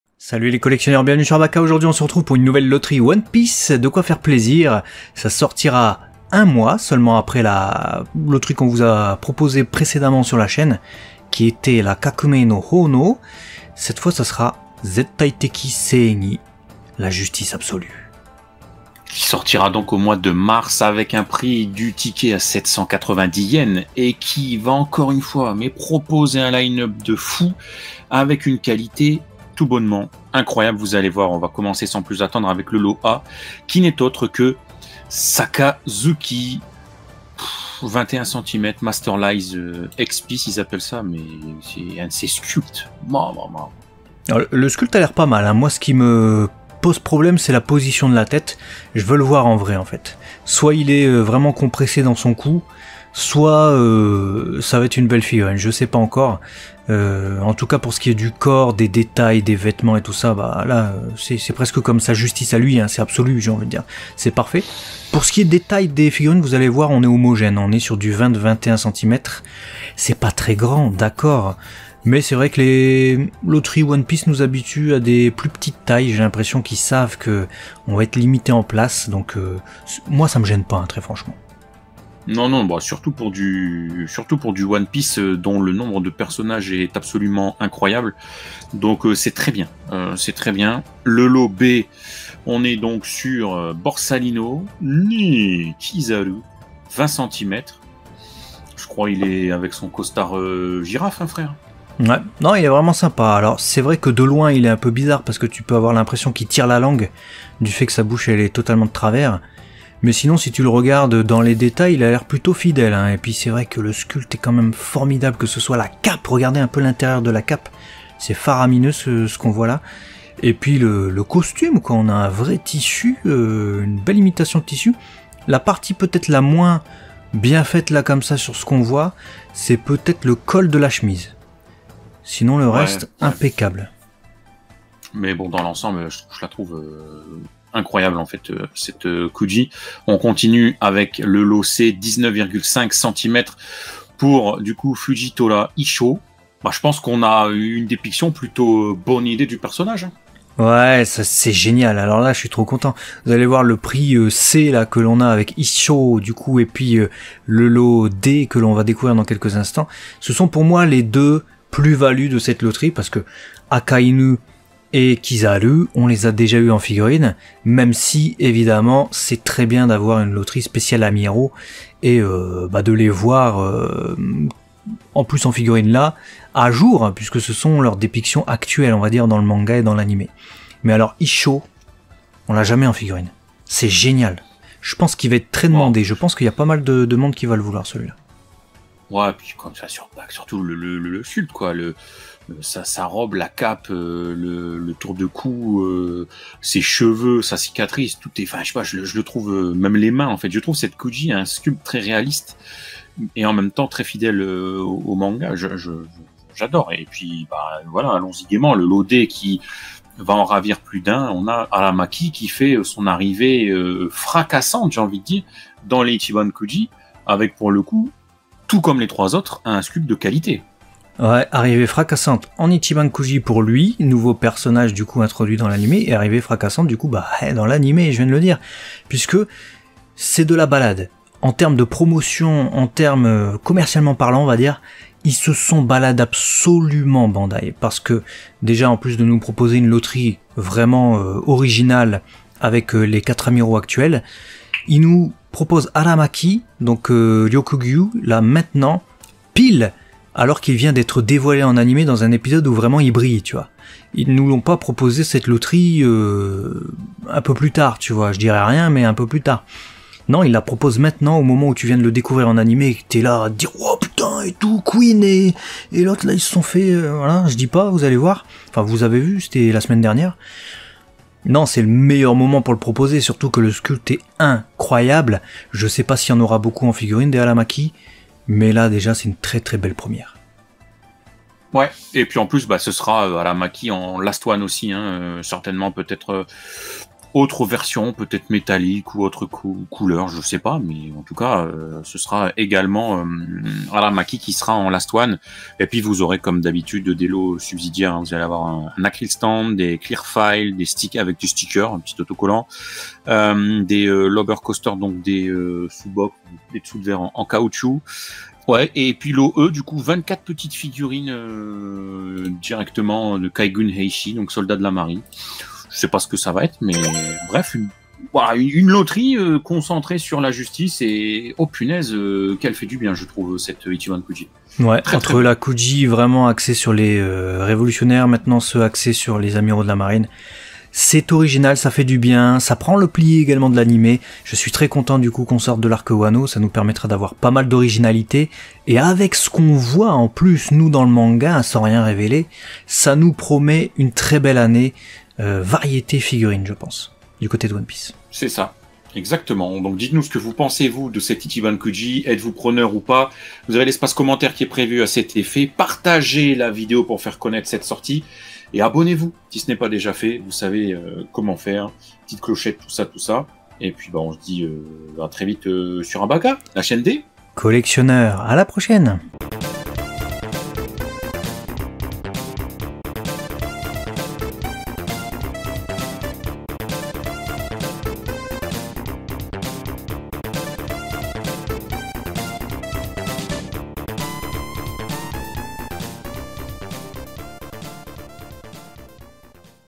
C'est la JUSTICE ABSOLUE !! One Piece Ichiban Kuji Zettai Teki Seigi | News
Une loterie spécialement dédiée à la justice cette fois-ci, C'EST BEAU !

[Credits]
►Video: Abaca Channel
►Edition: Max